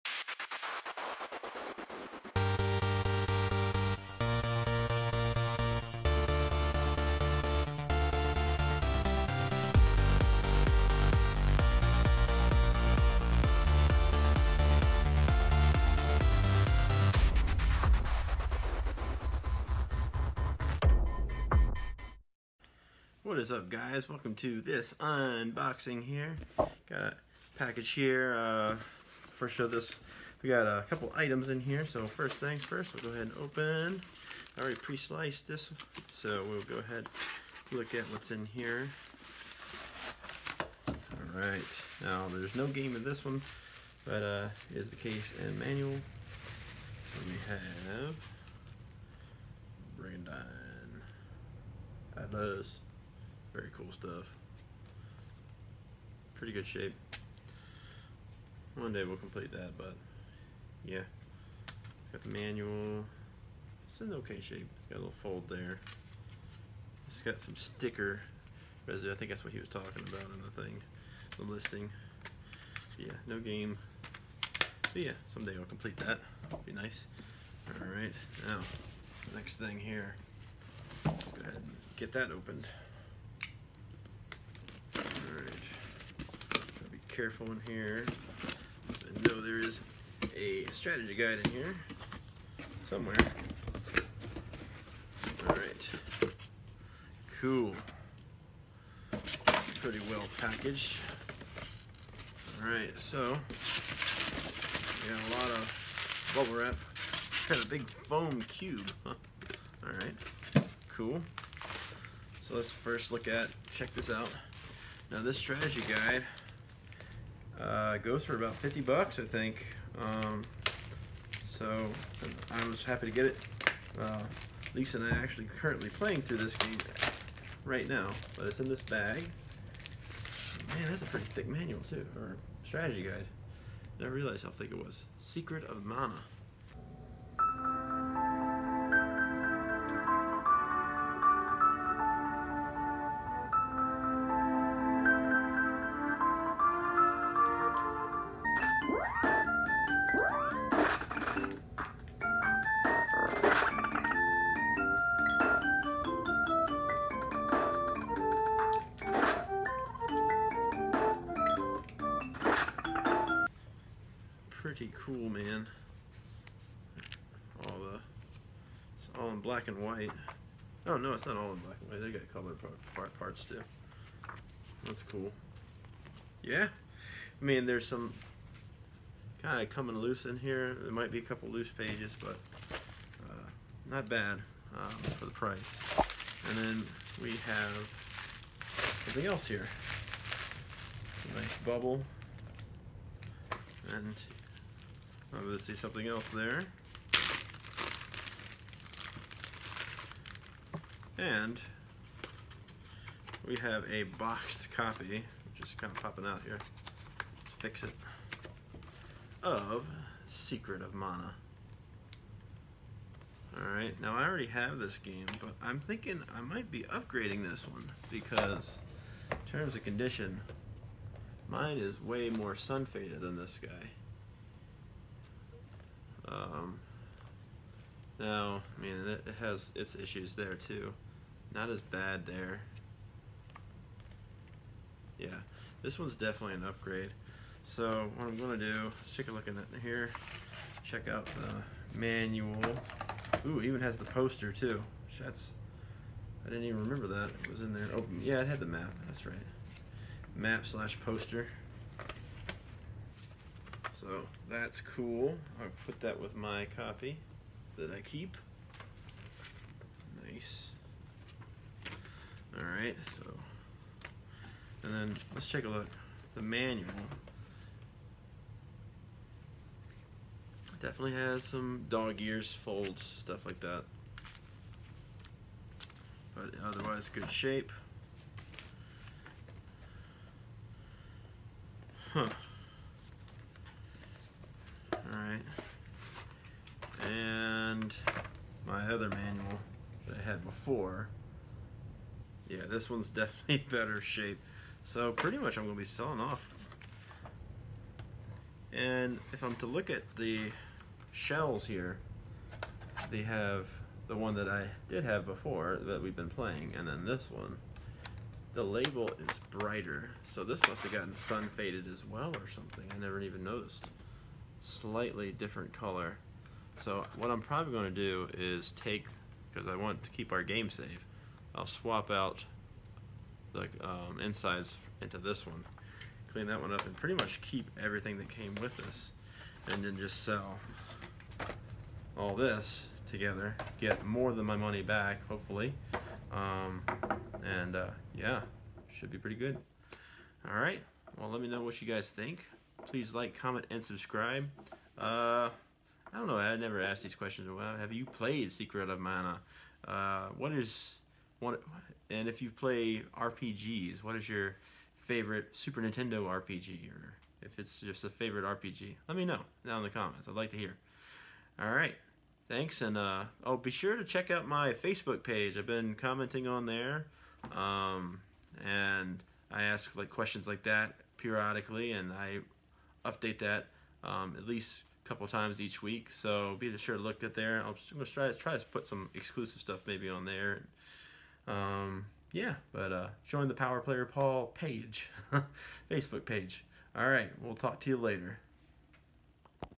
A Package For 3P "Brigandine and Secret of Mana"
It's RPG Mania once again here at the game room! This episode, I snag up deals on the case and manual to a rare Playstation RPG, Brigandine and a CIB copy of Secret of Mana complete with the strategy guide! Did I get a good deal? Let me know down in the comments along with what are some of your favorite RPG's (especially on the SNES!). Each week I post updates to the collection, polling questions and exclusive content!